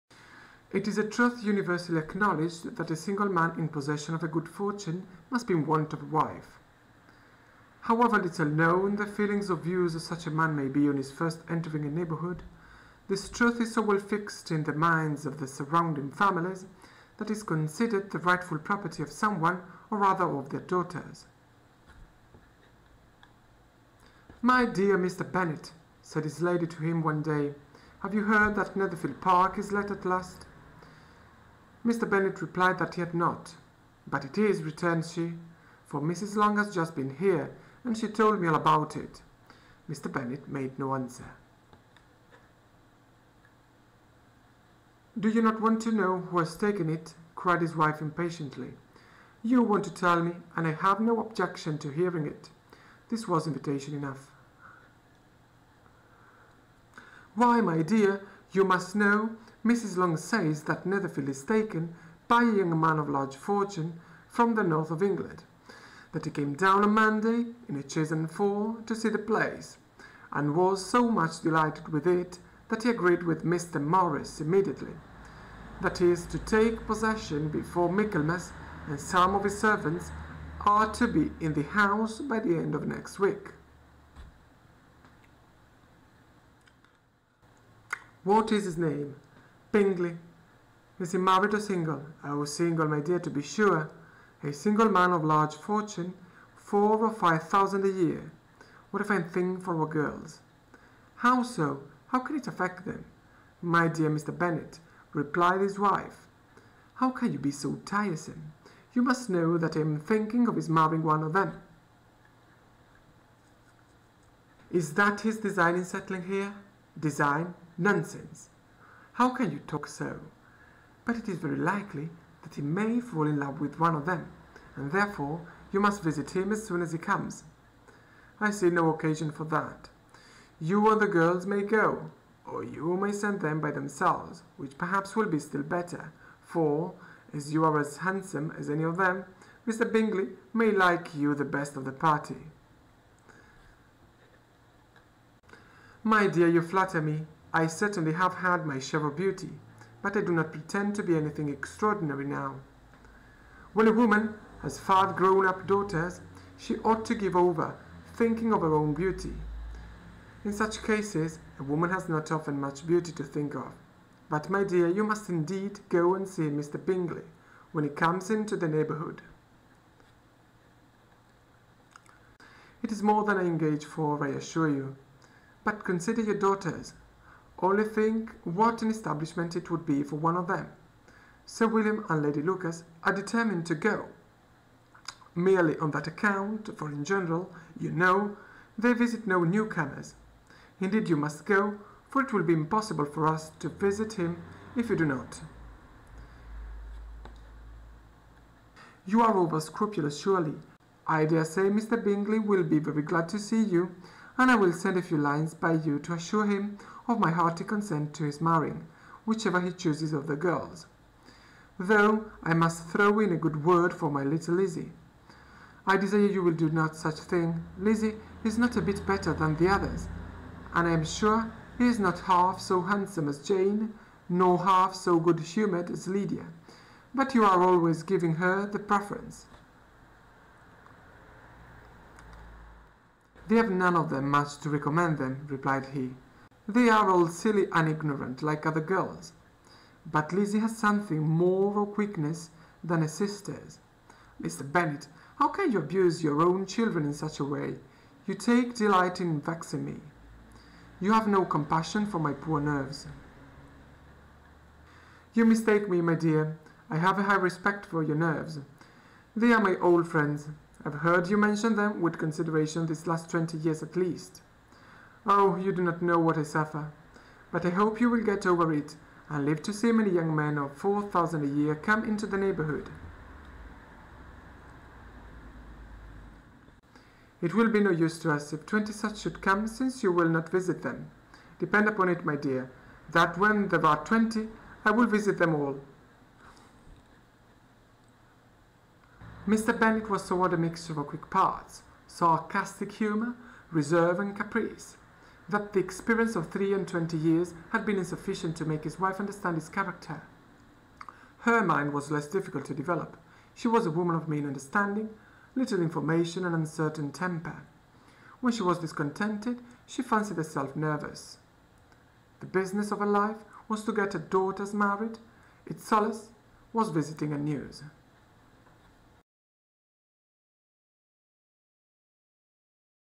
Pride and Prejudice - Chapter 1 - British Book - English Reading Skills - Audiobook - Accent
Jane Austin - Pride and prejudice